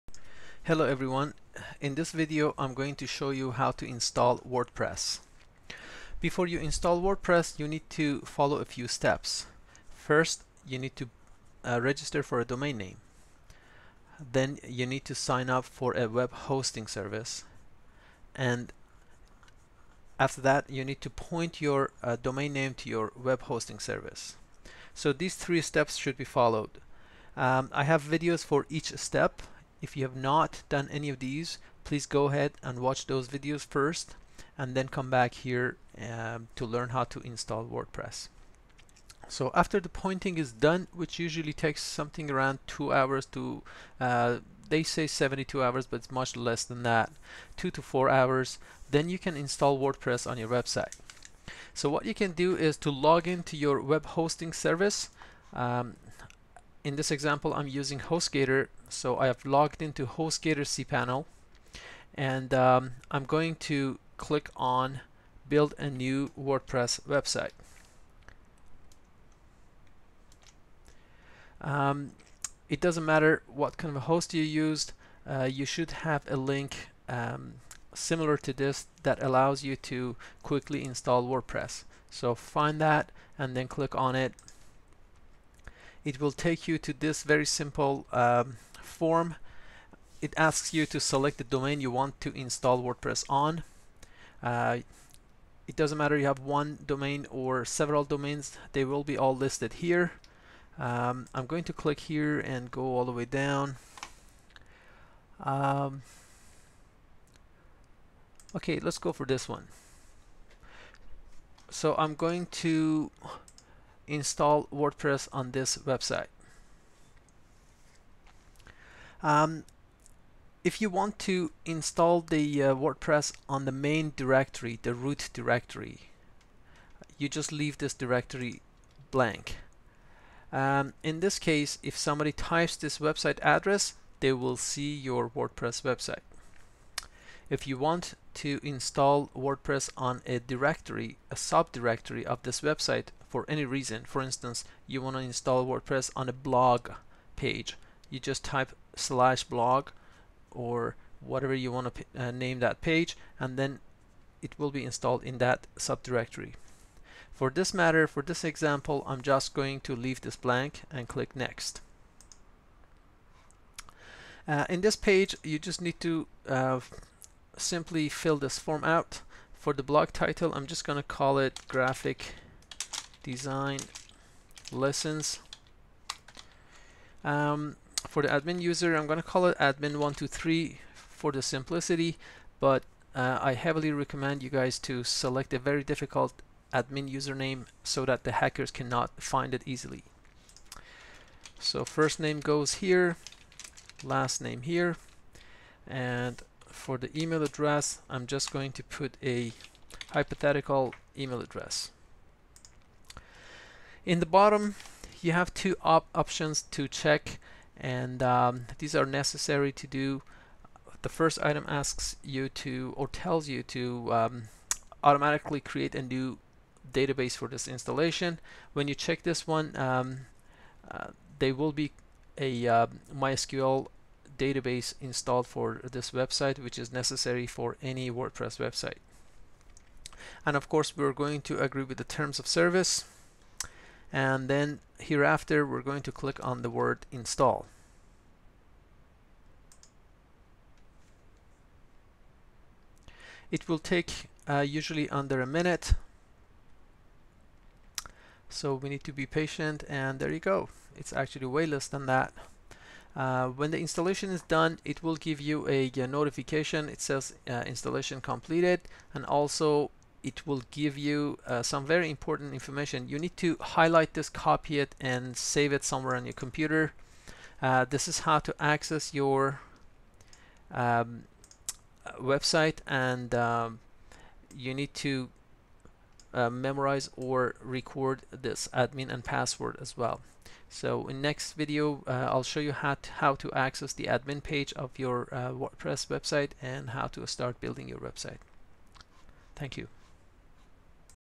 How to Install WordPress
How to Install WordPress. A step by step demonstration on WordPress installation on Hostgator cPanel.